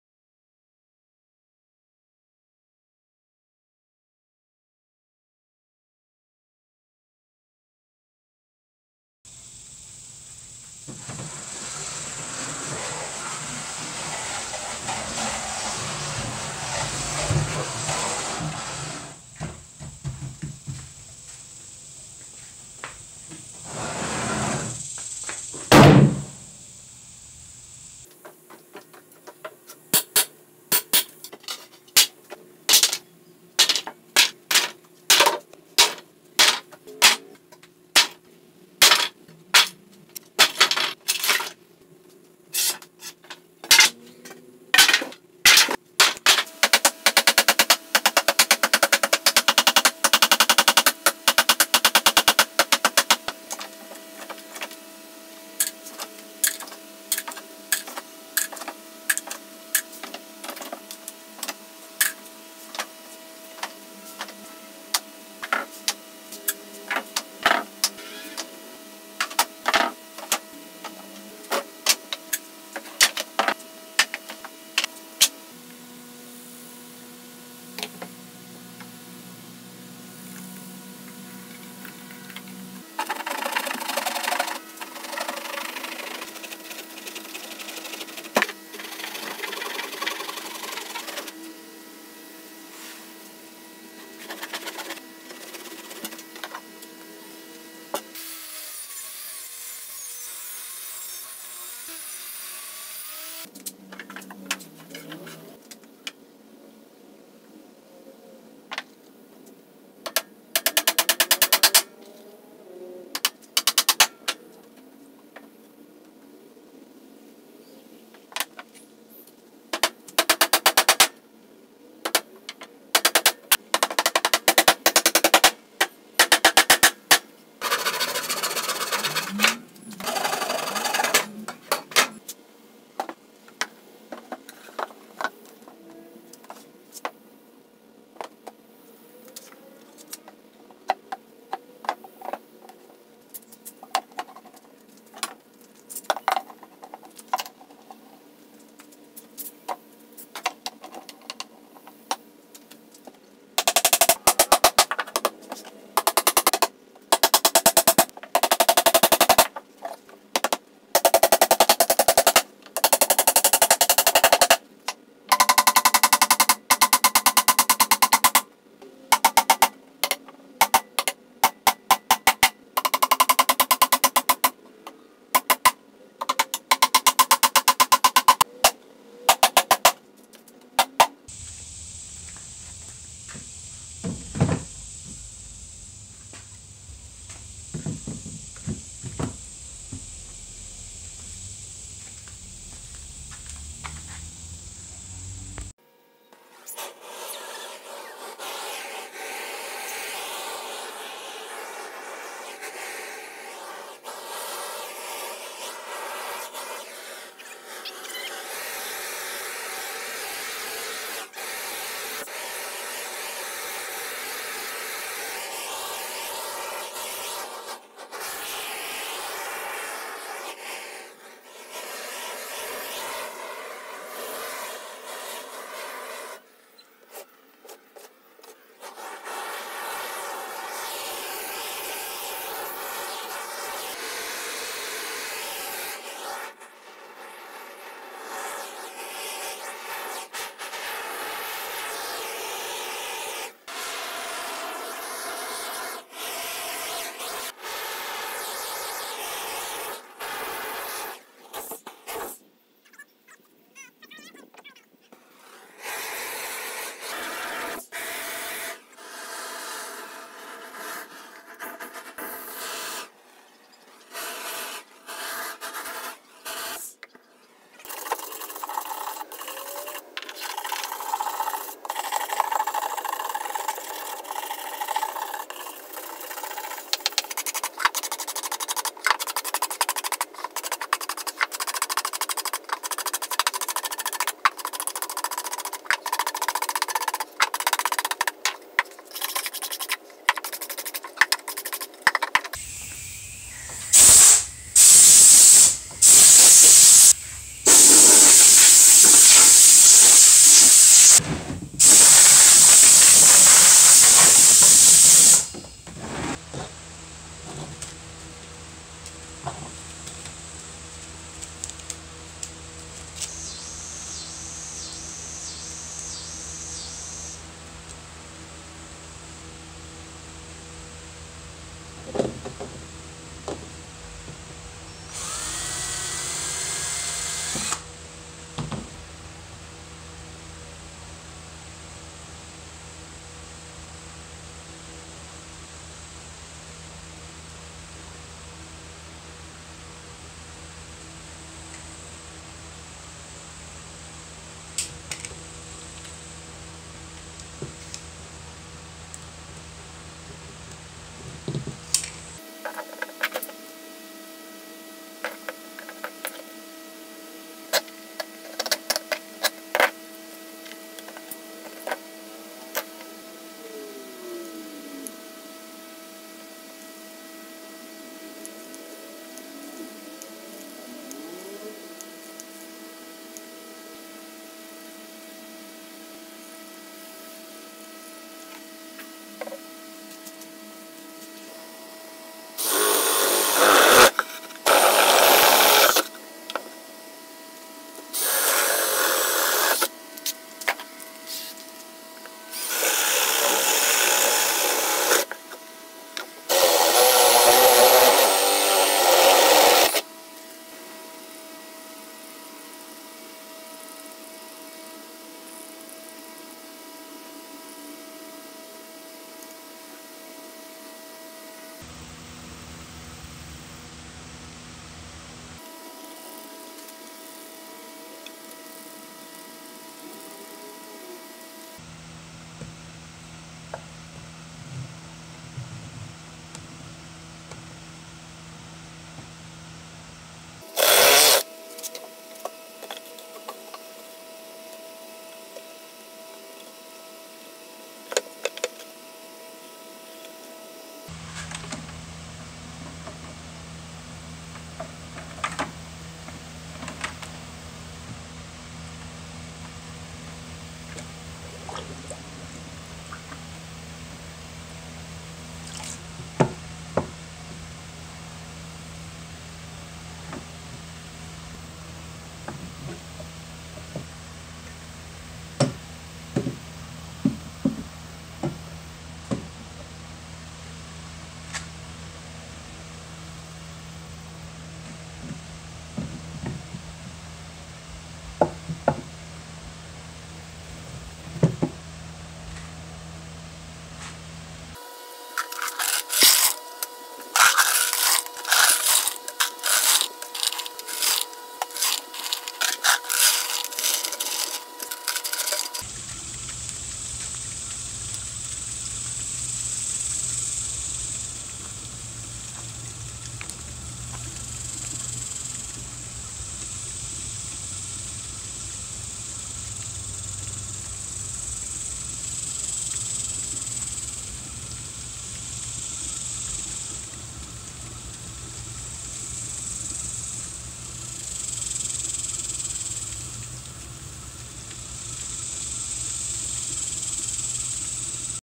Easy DIY Flaming Fountain From An Old Pallet
After I saw his video I decided to make one for myself from an old pallet. I bought the bucket tho.